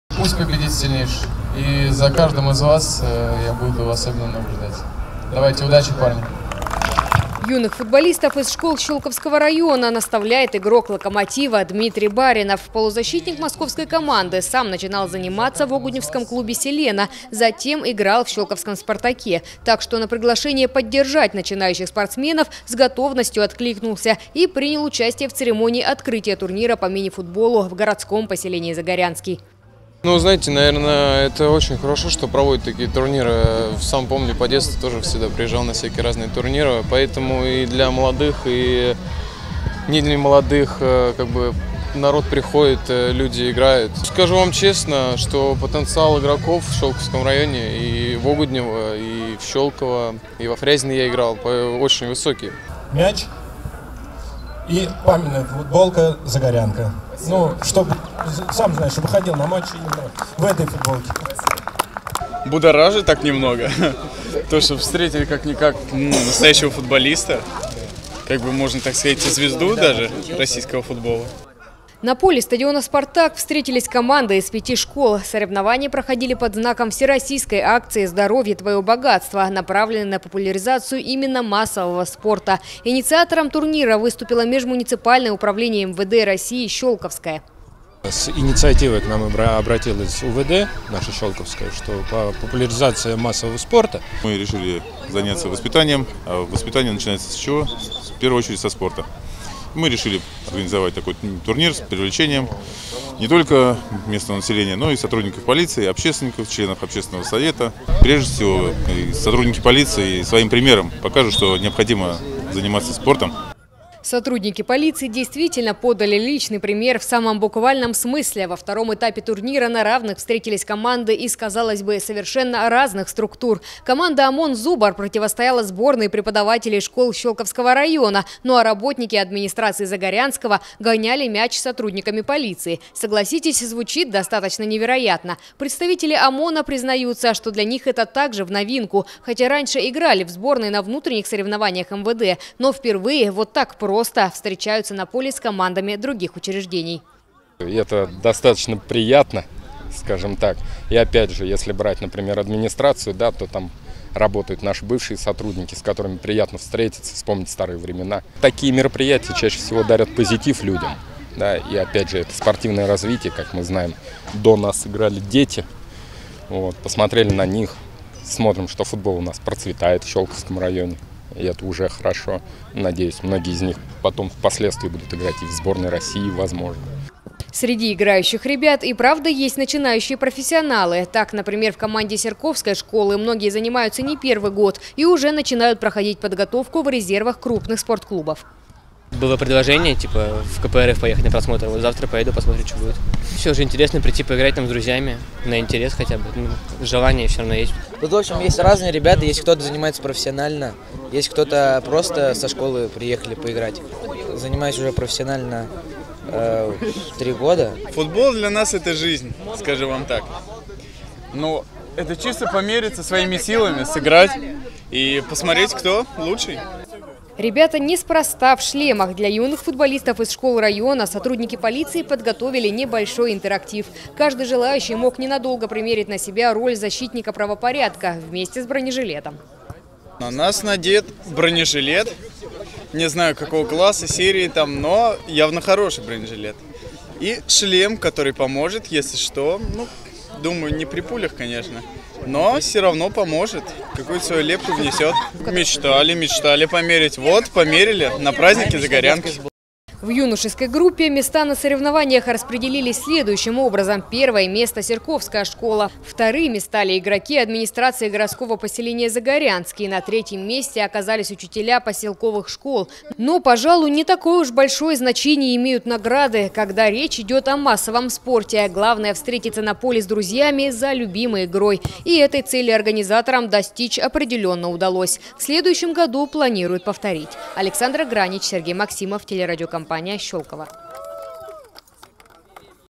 ОМОН «Зубр» сыграл в мини-футбол с преподавателями школ Щёлковского района
Турнир по мини-футболу прошёл в Загорянском. Команды ОМОН «Зубр», преподавателей школ района, Щёлковского отделения полиции и администрации поселения Загорянский встретились на поле стадиона «Спартак». В юношеской группе соревновались команды школ Щёлковского района. Инициатором соревнований в рамках всероссийской акции «Здоровье- твоё богатство» выступило межмуниципальное управление МВД России «Щёлковское».